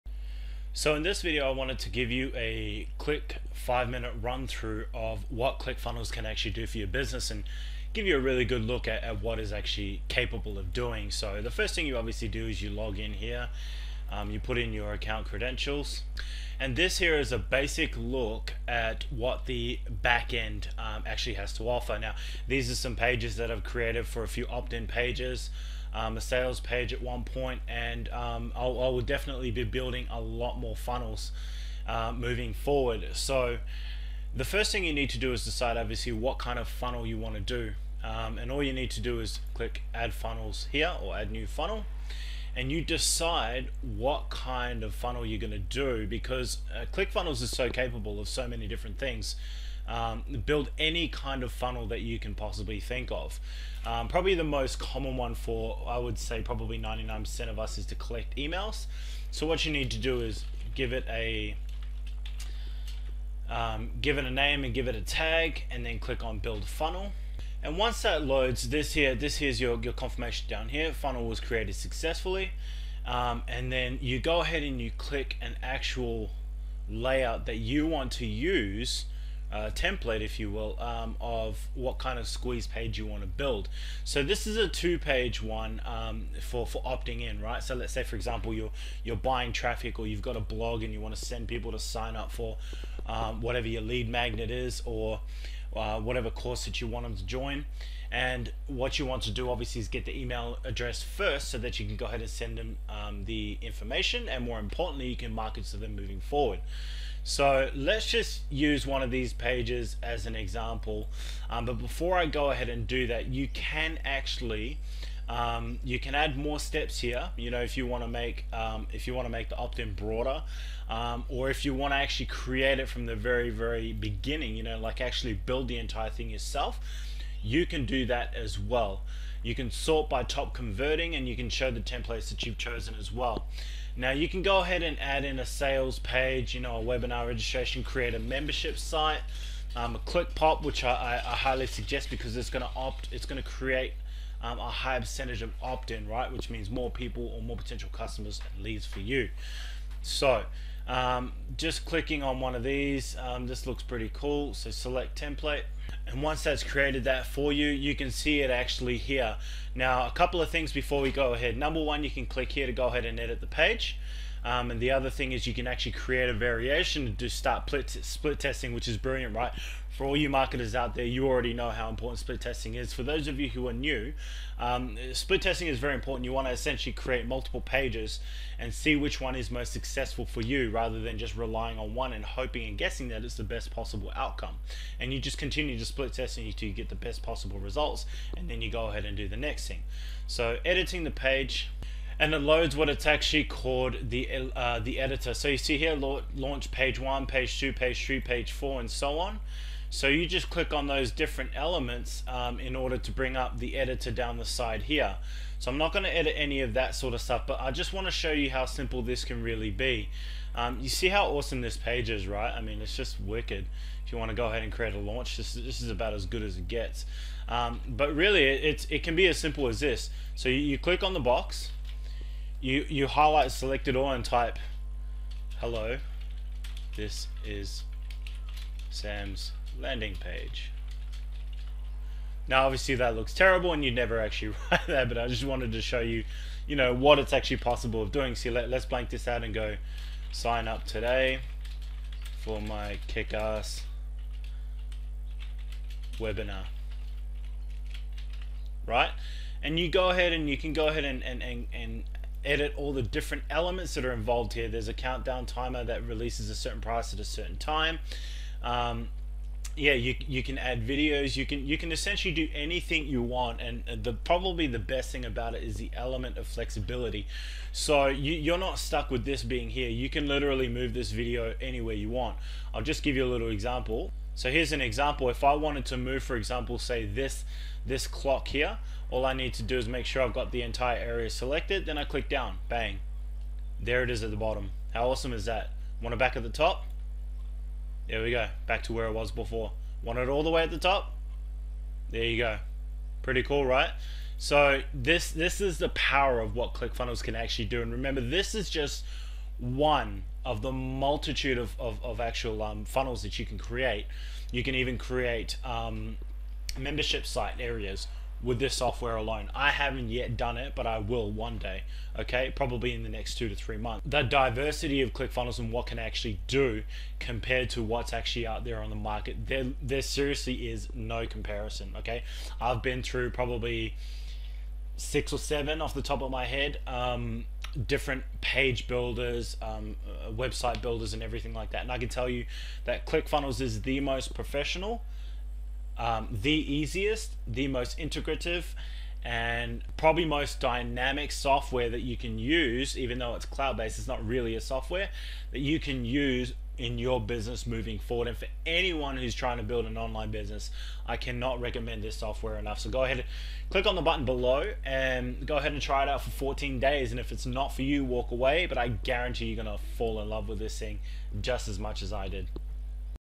A Look At Clickfunnels Features In Action
In This quick 5 minute video i give you a walk through of clickfunnels features & backend where you login & create the funnels of your choice!

I go over the in's & out's & provide one of the most in-depth looks at clickfunnels.

When you get into clickfunnels & its features you'll know exactly what to expect. And nothing will look unfamiliar to you.

00:25 First Look at Backend

00:40 Adding First Funnel

1:10 Templates to choose from

2:15 What else you can do with clickfunnels

2:40 A Few Things before you Edit

2:50 Why Split Testing is Important

3:13 First Look At Editor

3:40 Quick & Easy editing

4:35 How to Move ANYTHING in 1 click

5:20 Theres No Equal to Clickfunnels